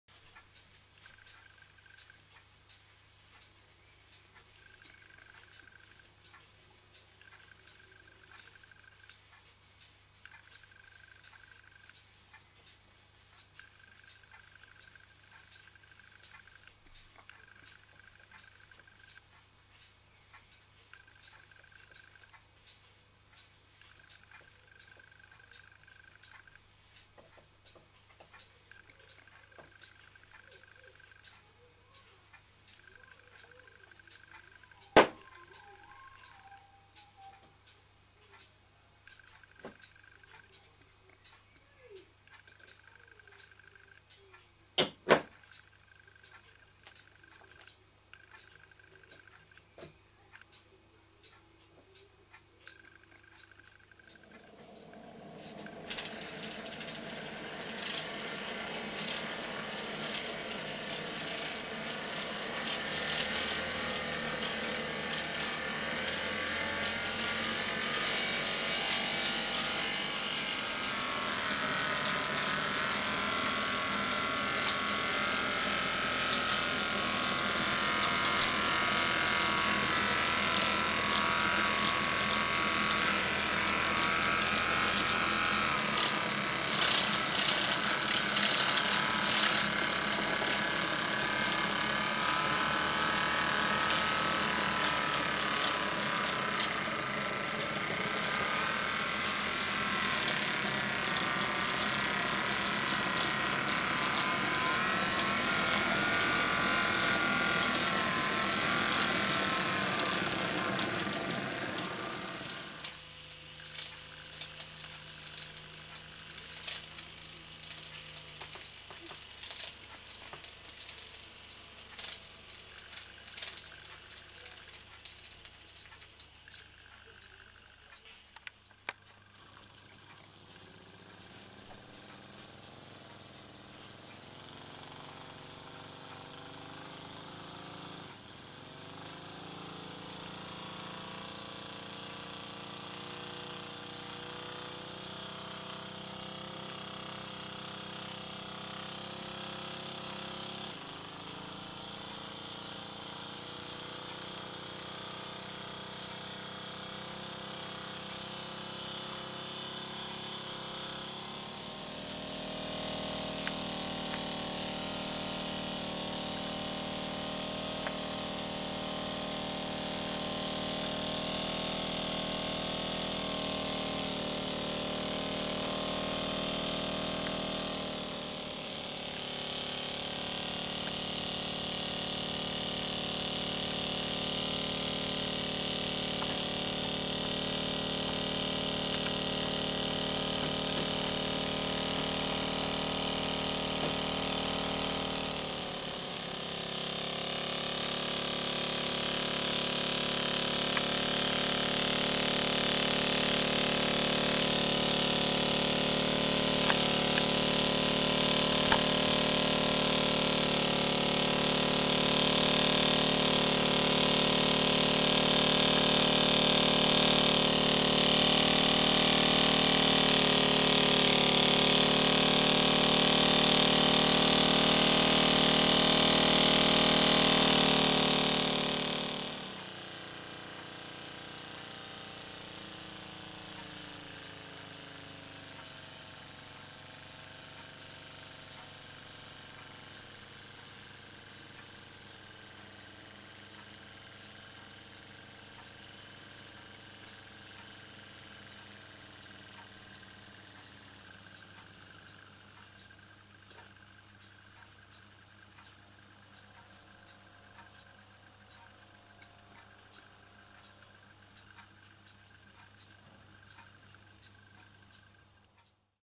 Peake V2 on steam & air
Peake Engines V2 steam engine running on steam & air.